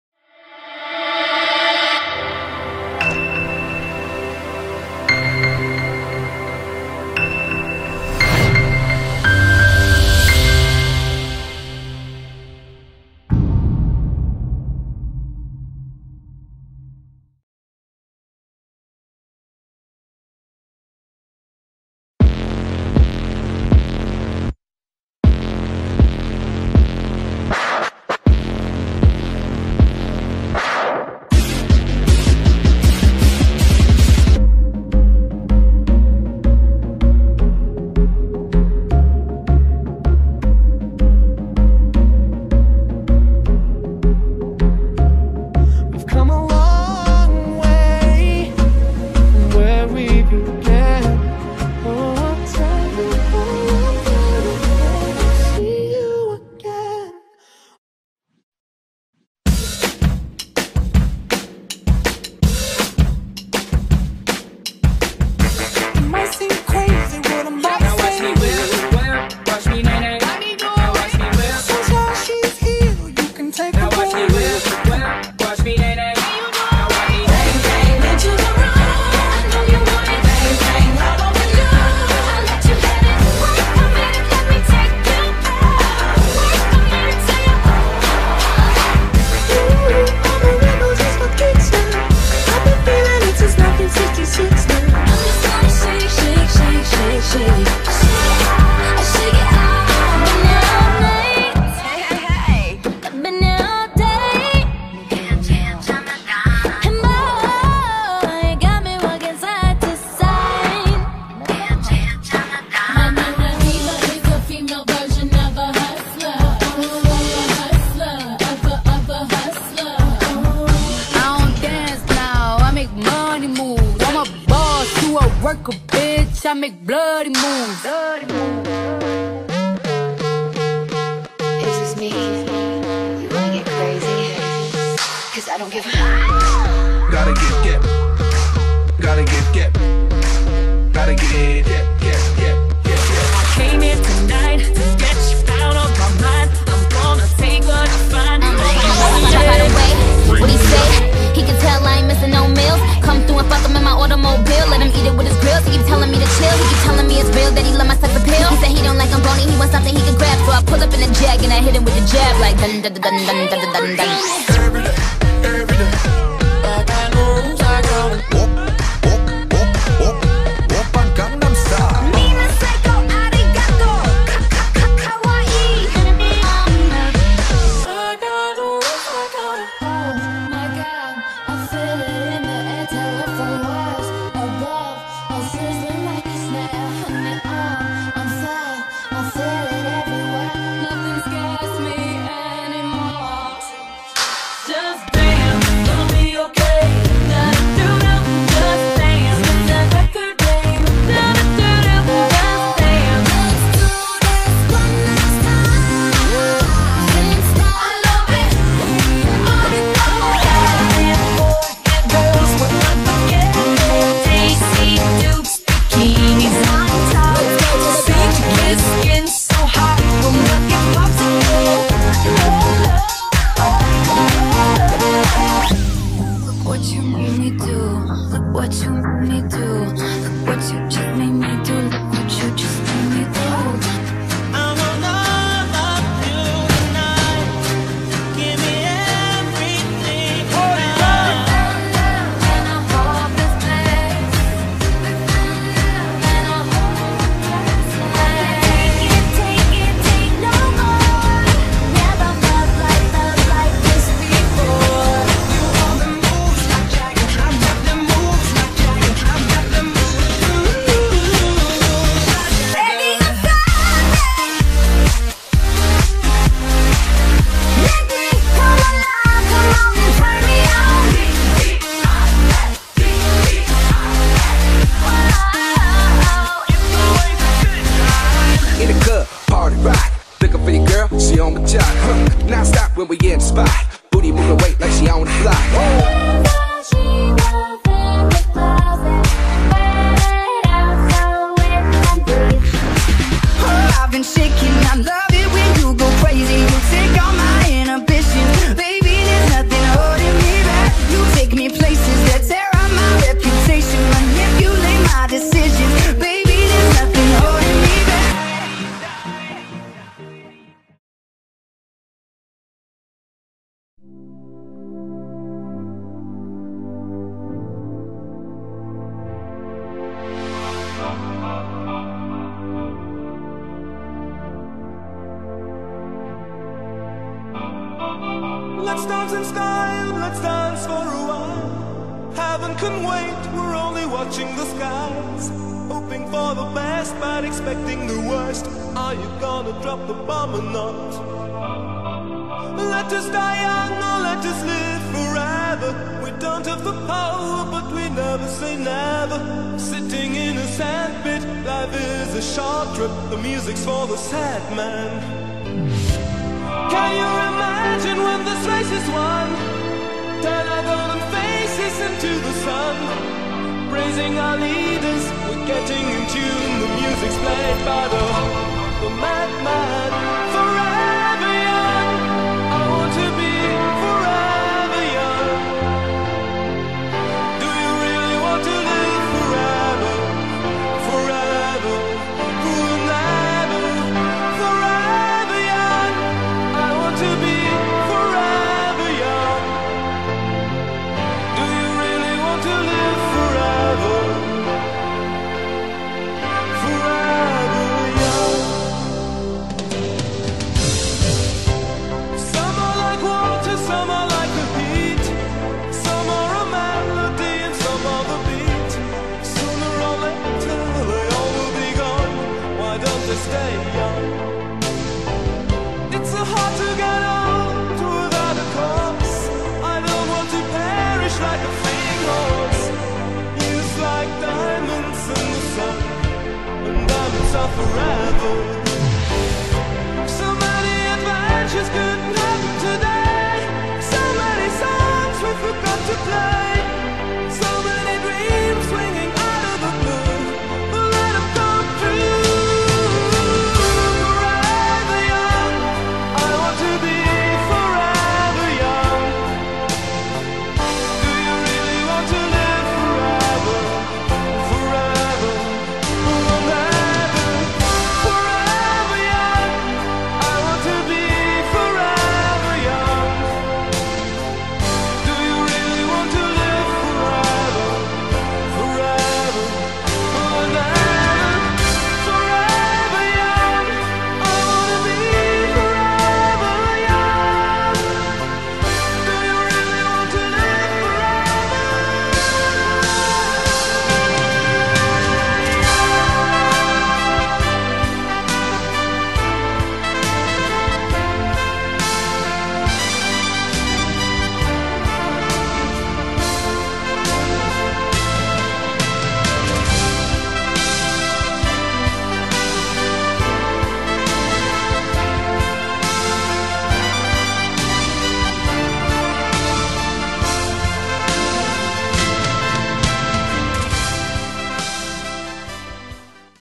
Seniors Show 2019 - Athens College
Οι μαθητές της Γ' Λυκείου του Κολλεγίου Αθηνών αποχαιρετούν τα μαθητικά χρόνια, περιμένοντας με ανυπομονησία να ξεκινήσουν ένα νέο κεφάλαιο στην ζωή τους.
Το βίντεο αυτό προβλήθηκε στις 14/4/2019 στα πλαίσια του διαχρονικού Πανηγυρίου του Κολλεγίου στο Show Λυκείου.